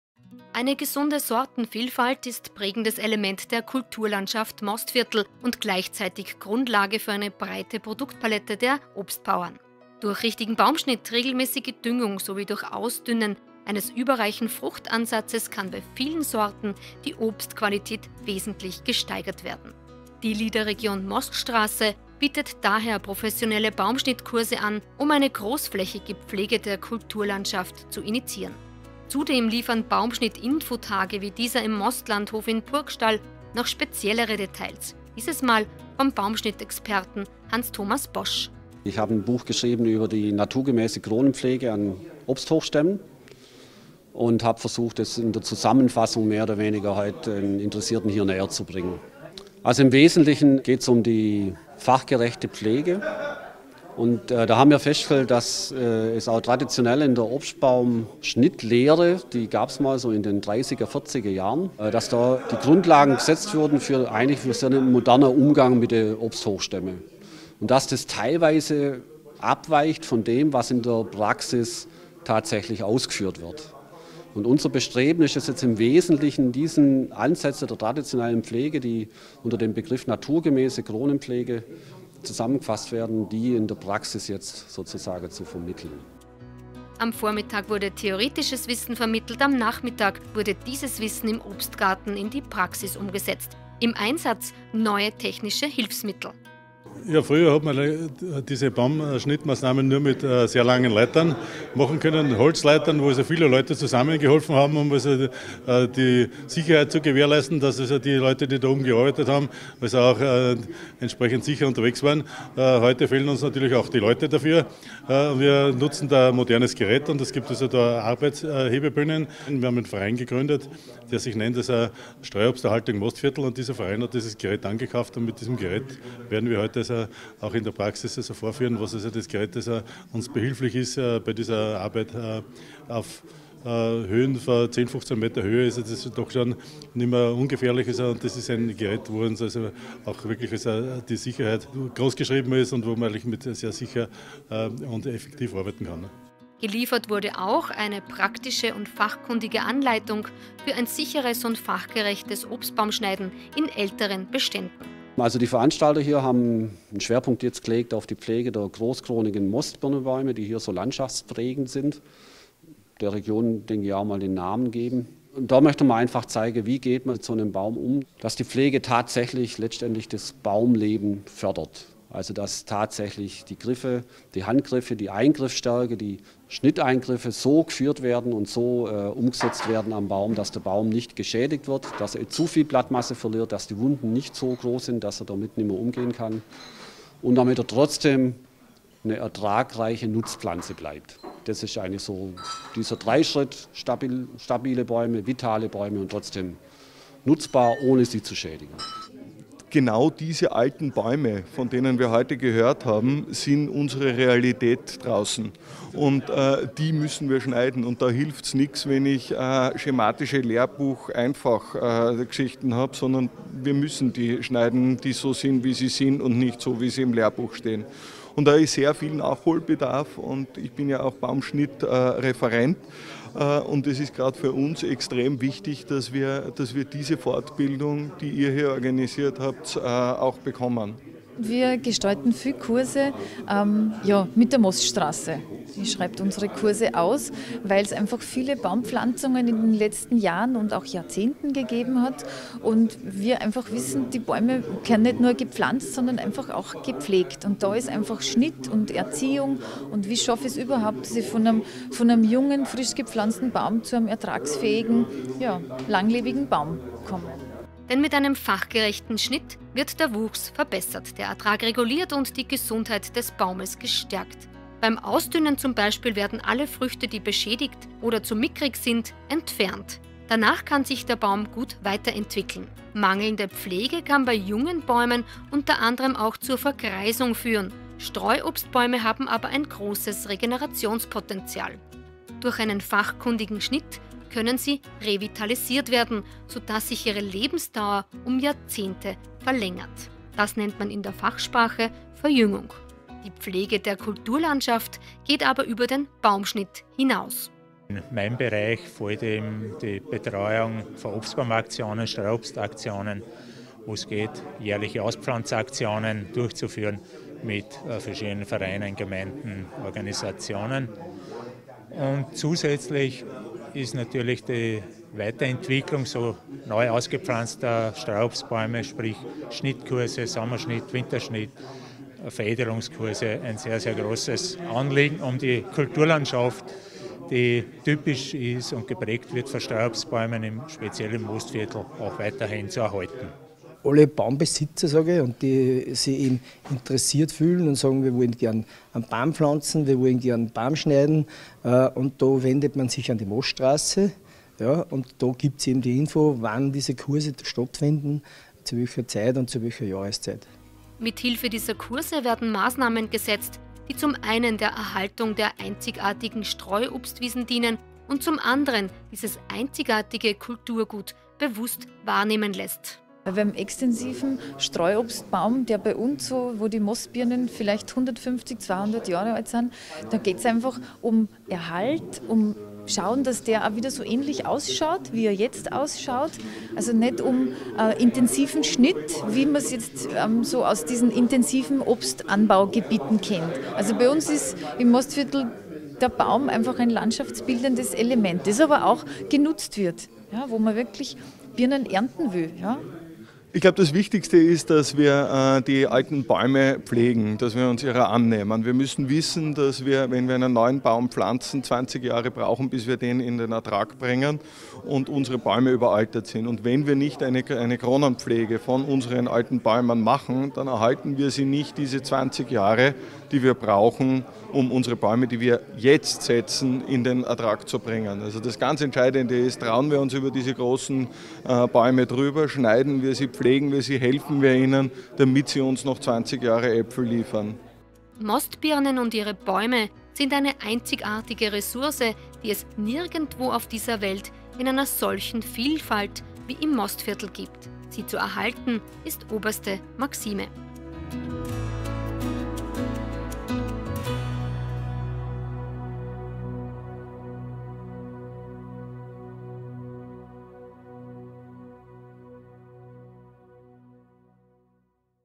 Baumschnitt Infotag
Eine gesunde Sortenvielfalt ist prägendes Element der Kulturlandschaft Mostviertel und Grundlage für eine breite Produktpalette der Obstbauern. Durch richtigen Baumschnitt, regelmäßige Düngung sowie durch Ausdünnen eines überreichen Fruchtansatzes kann bei vielen Sorten die Obstqualität wesentlich gesteigert werden.  Die Leader Region Moststraße bietet daher professionelle Baumschnittkurse an, um für eine großflächige Pflege der Kulturlandschaft zu initiieren.